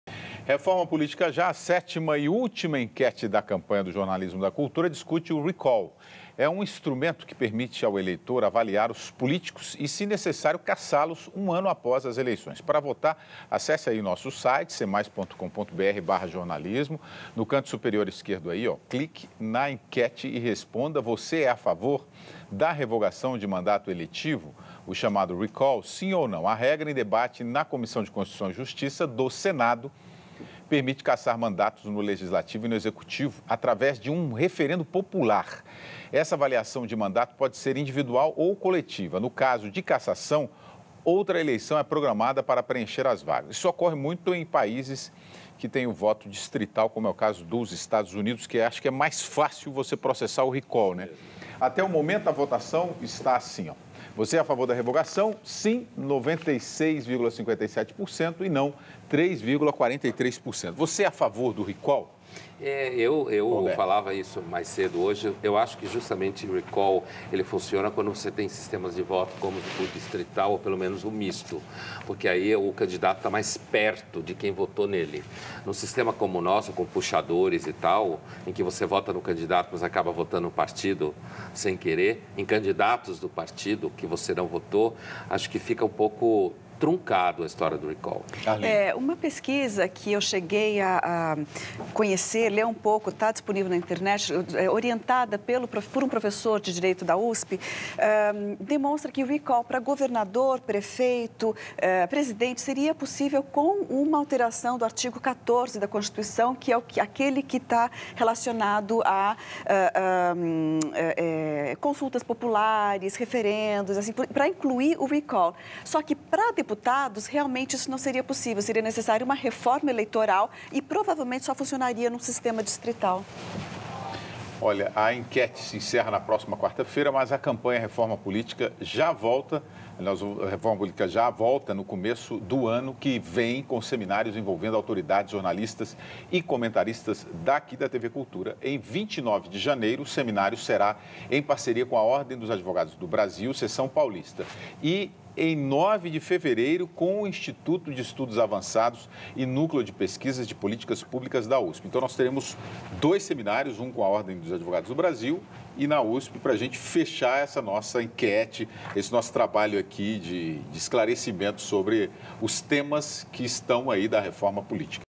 Luiz Pondé e Arlene Clemesha debatem sobre Recall de Políticos - 18/12/2014
Luiz Pondé e Arlene Clemesha debatem sobre Recall de Políticos e Reforma Política - 18/12/2014